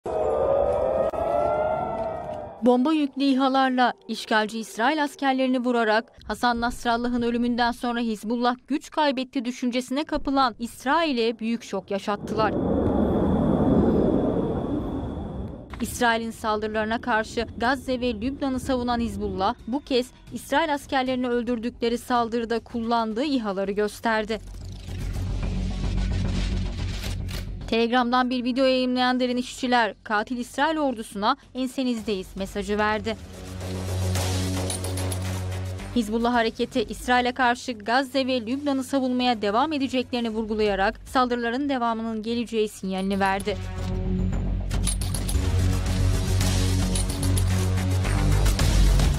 Hizbullah İHA’ları İsrail semalarında!
Ortadoğu’da tansiyon her geçen gün daha fazla artarken dün gece Hizbullah tarafından İsrail’in askeri üslerine yapılan saldırılarda çok sayıda İsrail askeri öldü ve yaralandı. Hizbullah saldırıyı gerçekleştirdiği İHA’larını gösterdiği bir video paylaştı.

İsrail’in soykırım uyguladığı Ortadoğu coğrafyasında savaş beklentileri yükselirken Hizbullah tarafından İsrail’in askeri üslerine saldırılar gerçekleştirildi.

Hizbullah’ın İHA’lar aracılığıyla gerçekleştirdiği saldırılarda çok sayıda İsrail askeri öldü ve yaralandı. Hizbullah bu saldırılarda kullandığı İHA’ları gösterdiği bir video yayınladı.